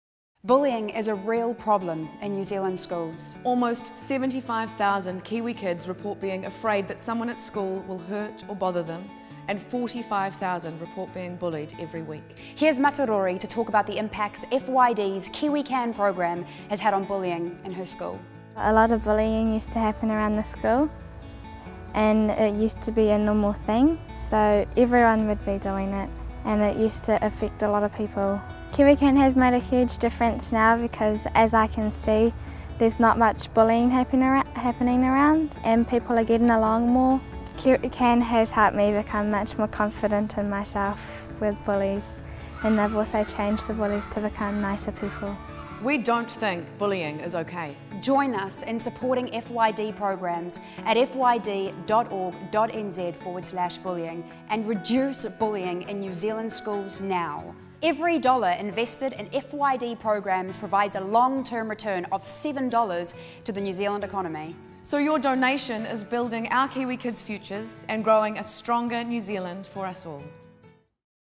Bullying Is not Okay - June 2015
Ria Vandervis and Kerry-Lee Dewing FYD Ambassadors talk about Bullying in NZ